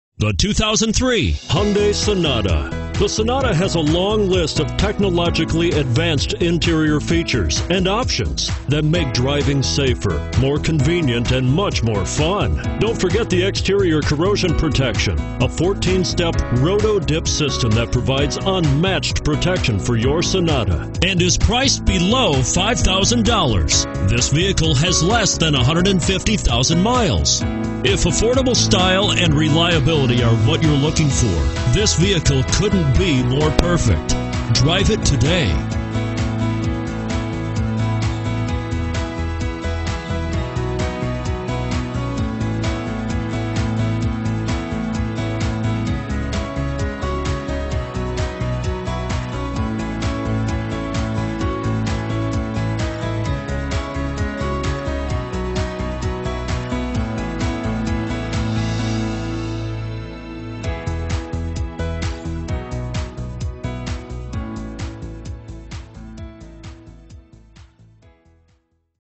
2003 Hyundai Sonata Golden Valley MN LJL7366B
2003 Hyundai Sonata

Clean Auto Check Local Trade, and No Accident History, Fuel Efficient. Here it is! No games, just business! brbrConfused about which vehicle to buy? Well look no further than this attractive 2003 Hyundai Sonata. J.D. Power and Associates gave the 2003 Sonata 4 out of 5 Power Circles for Overall Initial Quality. A spacious car that gets great fuel mileage... Why torture yourself driving a small commuter box up and down the highway every day when you can ride in roomy comfort. brbr